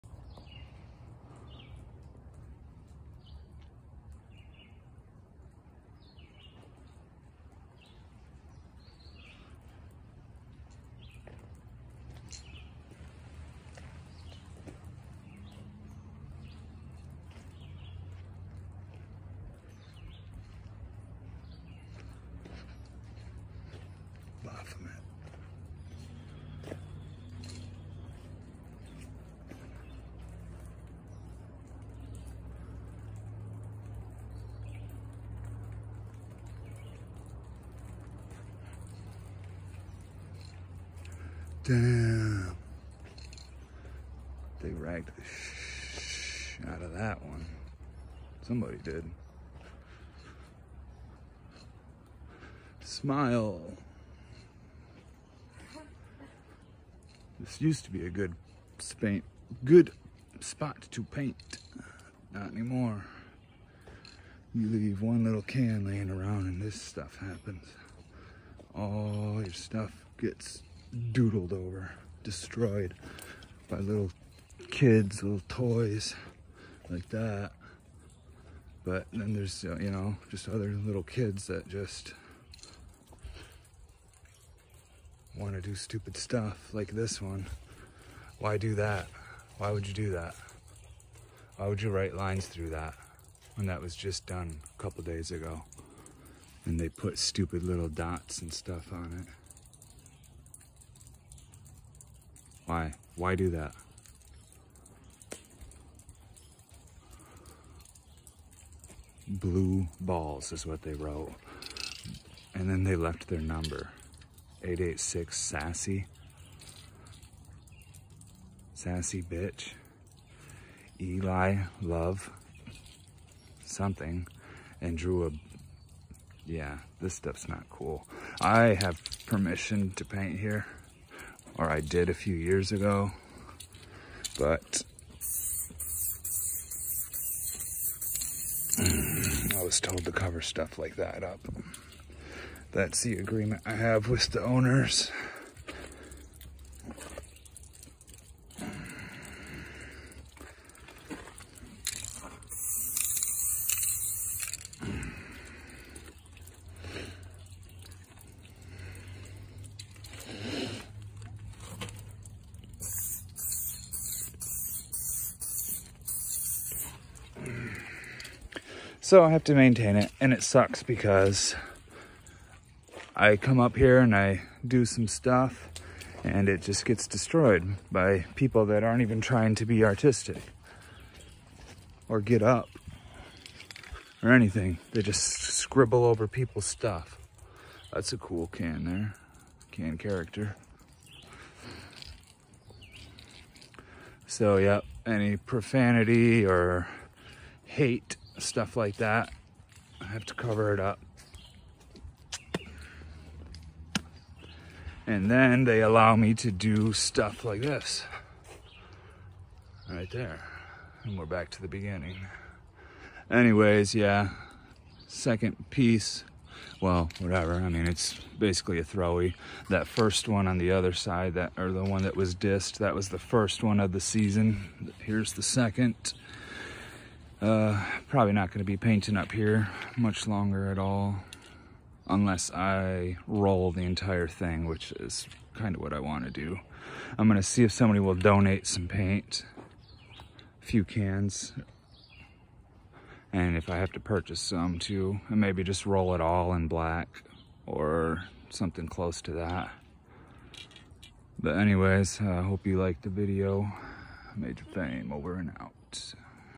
2022 Spring graffiti hunter Traverse City MI
A quick walk around at a local hidden chill spot near Munson hospital in TC MI